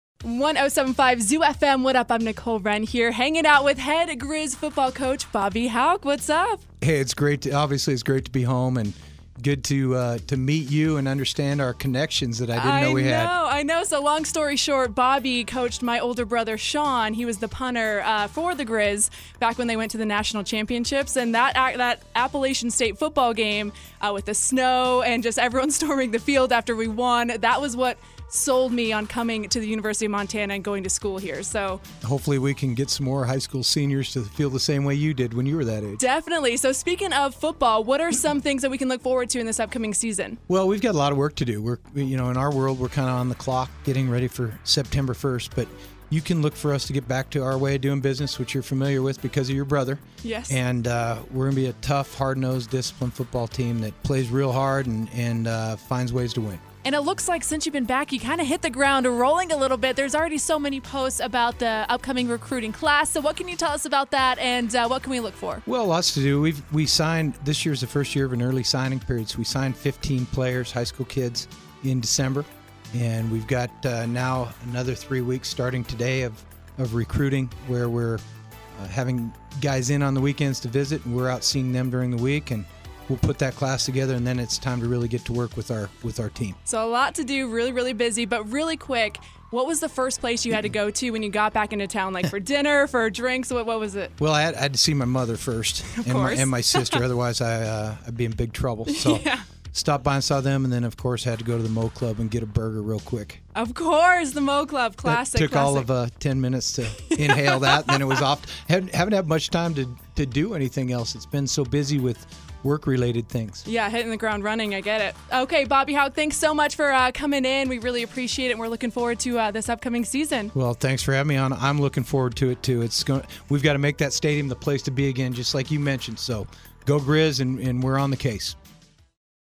Exclusive Interview with Griz Head Coach Bobby Hauck
Nicole has a chat with University of Montana Grizzlies head coach Bobby Hauck.